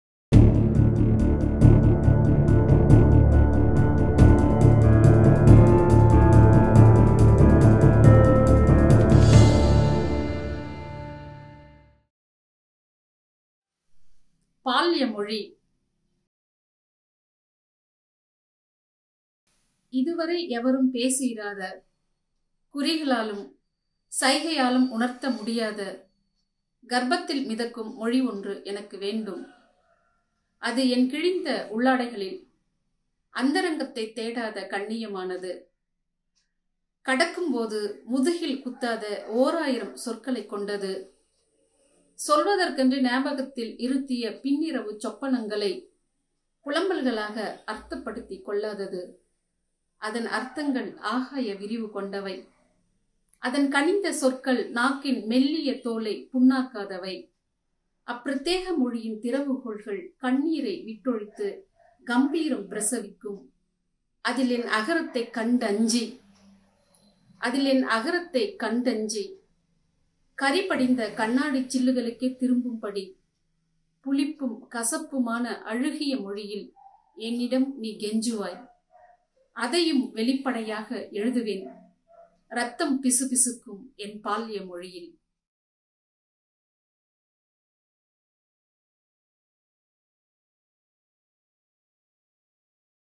Sukirtharani reads her poem 'Infant Language'
Sukirtharani grew up in one of the ten dalit families of a village called Lalapet in the Vellore district of Tamil Nadu. Her grandfather played the parai and her father worked as a labourer. As a child, she wasn’t very comfortable with her identity but she turned all her thoughts and questions into poetry.

Today, she is a fierce and fearless dalit feminist poet. The female body in her poems map her dalit identity. “A feminine body is disrespected, but a dalit feminine body is almost hated.” she said in an interview with The Wire. This intersectionality of caste and gender plays an integral role in her writing.

This week for Bol, Sukirtharani reads her poem, “Infant Language”, translated into English by Lakshmi Holmström.